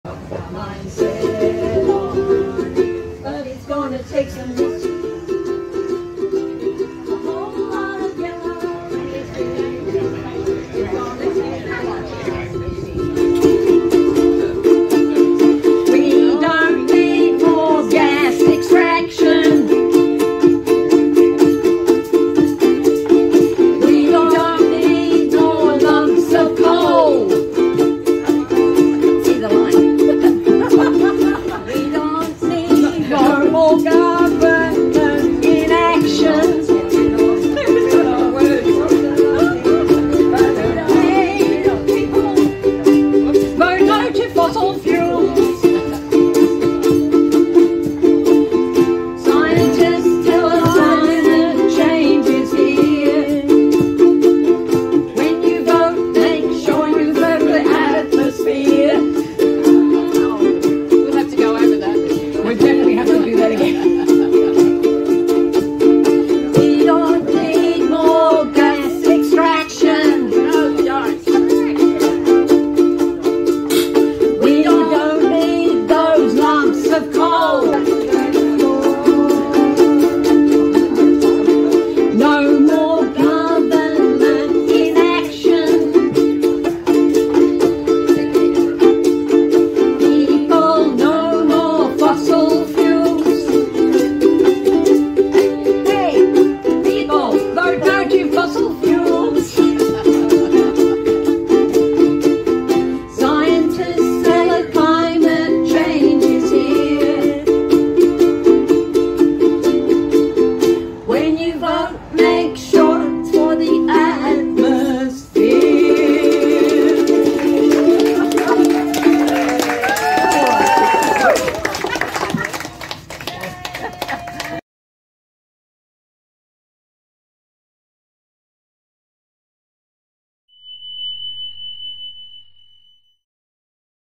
The Knitting Nana's @ Trees not Bombs 💞
The Knitting Nana's at Trees not Bombs.
The Knitting Nana's are a climate change activist group.
The played a major part in keeping The Northern Rivers CSG FREE.
EVERY ONE LOVE A NANA.